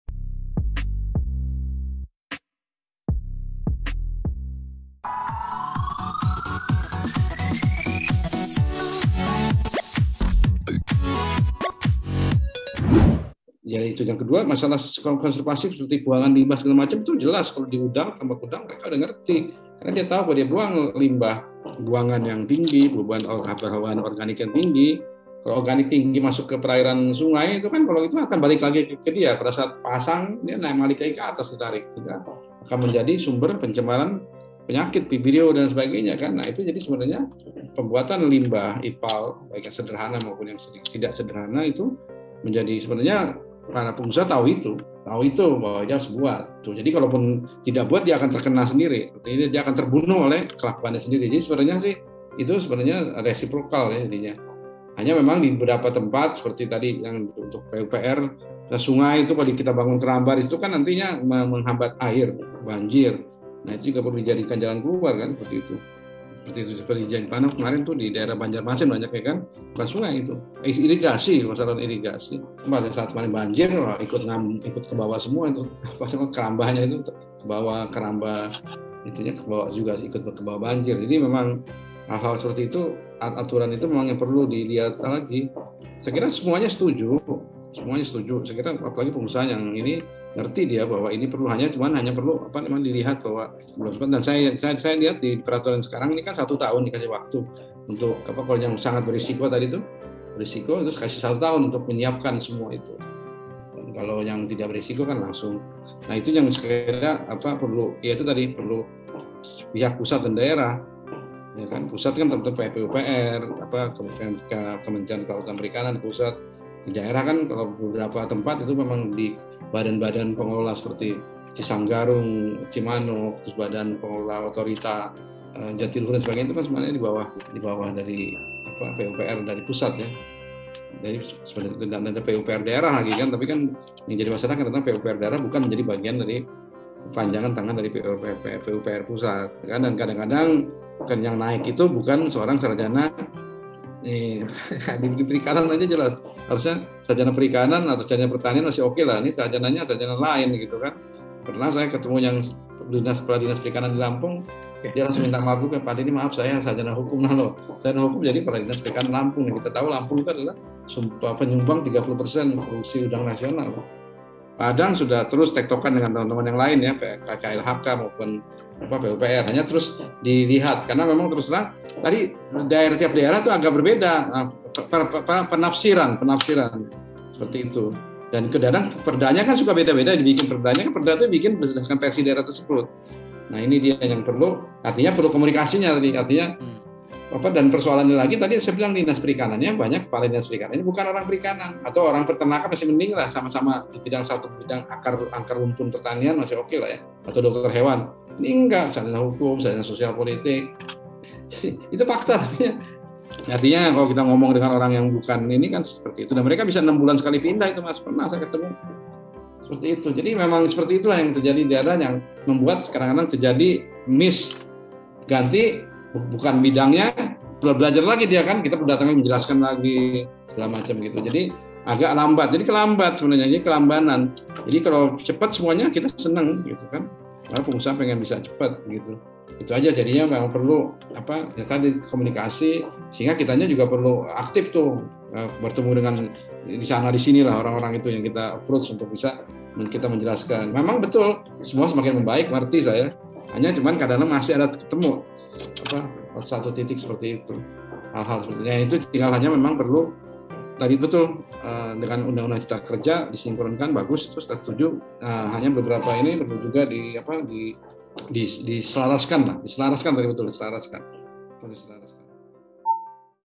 Sinkronisasi Regulasi: Optimalisasi Produksi dan Lingkungan | Denny Indradjaja (Sekjen MAI)
Terkait konservasi, pembuangan limbah jelas bahwa ditambak udang akan mengerti karena jika membuang limbah dengan beban pembuangan yang tinggi maka...

Video ini diambil pada acara Webinar PATAKA 70, dengan tema "BICARA REGULASI, ANTARA KONSERVASI DAN BISNIS PERAIRAN".

Acara dilaksanakan pada Kamis, 17 Maret 2022.

Kritik dan saran, silahkan disampaikan melalui kolom komentar di bawah video.

SEKRETARIAT:
Cikeas Country Residence D1 No. 10 Bojong Nangka
Gunung Putri, Kab. Bogor - 18963

TELEPON:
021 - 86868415